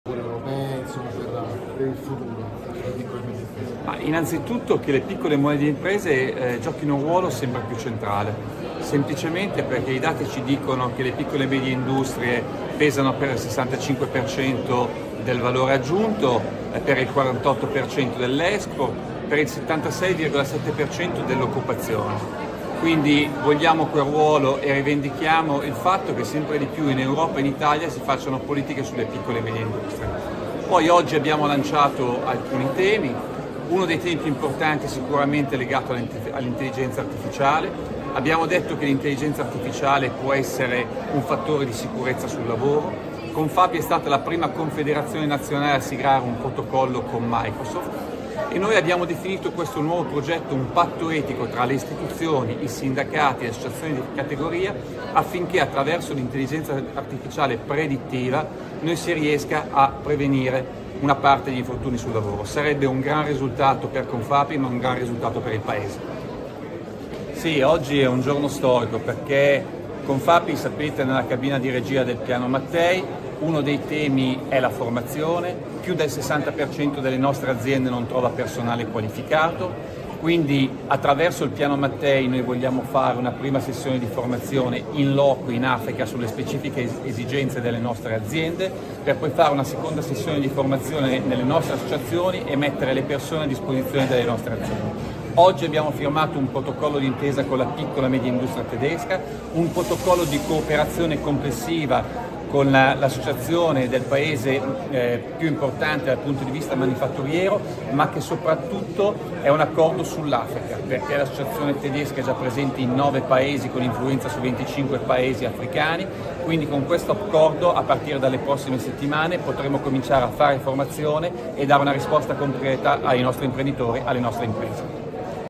Camisa (Confapi): "Le pmi giochino un ruolo sempre più centrale"
"Dalle piccole e medie industrie per il 65% del valore aggiunto"

POLITICA (Roma). "Le piccole e medie industrie giochino un ruolo sembra più centrale". Lo ha detto il presidente di CONFAPI, Cristian Camisa, al termine dell'Assemblea della Confederazione. "I dati ci dicono che le piccole e medie industrie pesano per il 65% del valore aggiunto, per il 48% dell'export, per il 76,7% dell'occupazione, quindi vogliamo quel ruolo e rivendichiamo il fatto che sempre di più, in Europa e in Italia, si facciano politiche sulle piccole industrie", aggiunge Camisa. "Oggi abbiamo lanciato anche un tema, uno dei temi più importanti sicuramente legato all'intelligenza artificiale. Abbiamo detto che l'intelligenza artificiale può essere un fattore di sicurezza sul lavoro. CONFAPI è stata la prima confederazione nazionale a siglare un protocollo con Microsoft e noi abbiamo definito questo nuovo progetto un patto etico tra le istituzioni, i sindacati e le associazioni di categoria affinché, attraverso l'intelligenza artificiale predittiva, si riesca a prevenire una parte di infortuni (Stefano Chianese/alanews)

innanzitutto che le piccole medie imprese Ciao Pino vuole sempre più centrale semplicemente perché i dati ci dicono che le piccole beghe pesano appena 65% del valore aggiunto è per il 48% del esco per il 76,7% dell'occupazione Quindi vogliamo ruolo eventi chiamo il fatto che sempre di più in Europa e in Italia si facciano politiche sulle piccole medie Poi oggi abbiamo lanciato alcuni temi uno dei temi più importanti sicuramente legato all'intelligenza artificiale abbiamo detto che l'intelligenza artificiale può essere un fattore di sicurezza sul lavoro con Fabio è stata la prima Confederazione Nazionale ha siglato un protocollo con Michael e noi abbiamo definito questo nuovo progetto un patto etico tra le istituzioni i sindacati e associazioni di categoria affinché attraverso l'intelligenza artificiale predittiva non si riesca a  prevenire una parte gli infortuni sul lavoro sarebbe un gran risultato per comprato i manga risultato per il paese Sì oggi è un giorno storico perché con Papi sapete nella cabina di via Del Piano Mattei uno dei temi è la formazione più del 60% delle nostre aziende non trova personale qualificato quindi attraverso il piano Mattei noi vogliamo fare una prima se di formazione in loco in Africa sulle specifiche esigenze delle nostre aziende per postare una seconda sessione di formazione delle nostre associazioni e mettere le persone disposizione dei Rotterdam Oggi ho firmato un protocollo d'Intesa con la Piccola Media Industria Tedesca un protocollo di Cooperazione compressiva con l'associazione del paese più importante al punto di vista manifatturiero ma che soprattutto è un accordo sull'africa perché l'associazione tedesca è già presente in 9 Paesi con influenza su 25 paesi africani quindi con questo accordo a partire dalle prossime settimane potremmo  informazione e dà una risposta concreta ai nostri imprenditori alle nostre imprese